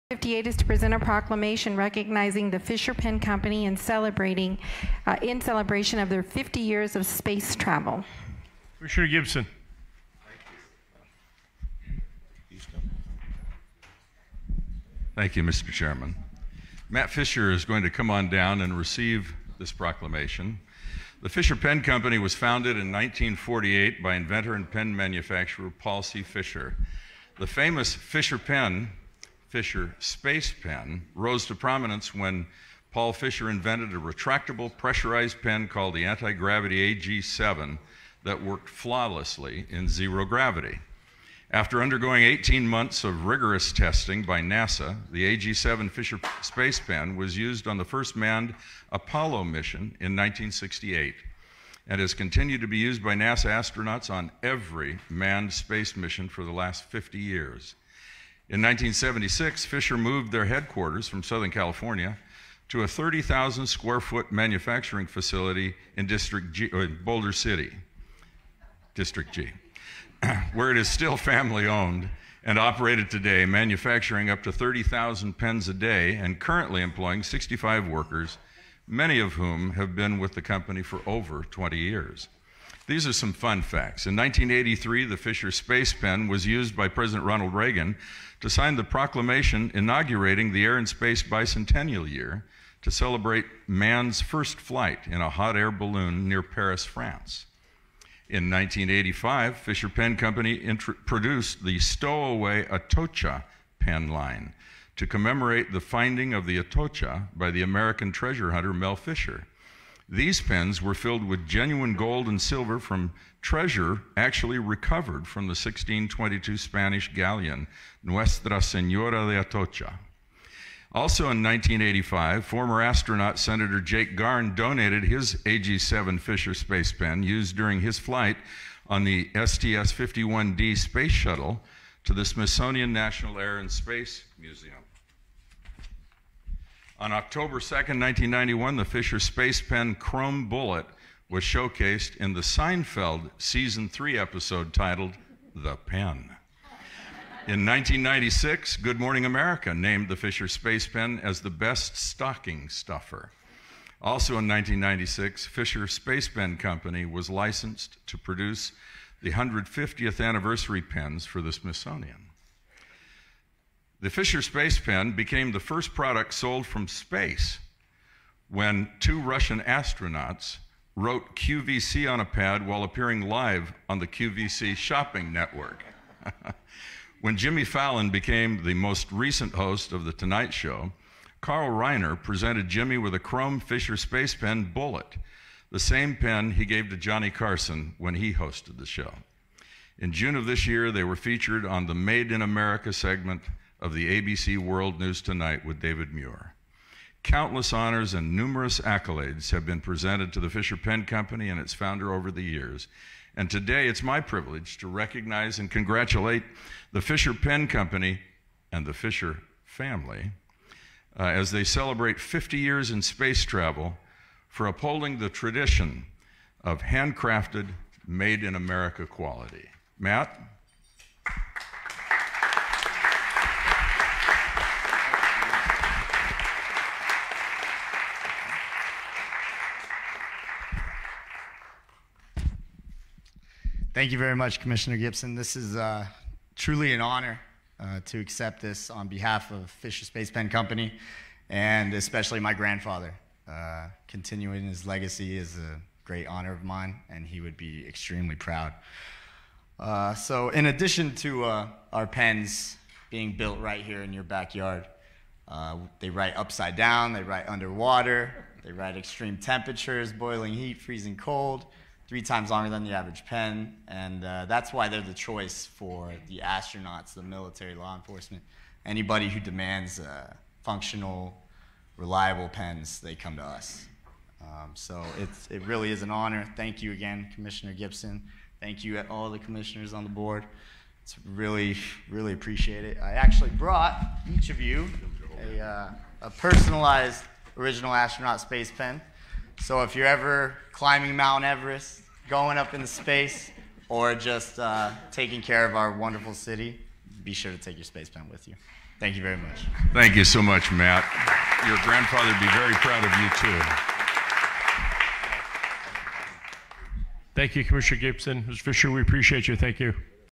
Fisher Space Pen receives Proclamation from Clark County Commissioner
On July 17, 2018 Matt Fisher of Fisher Space Pen was presented with a proclamation celebrating 50 years in space from the Clark County Commissioners.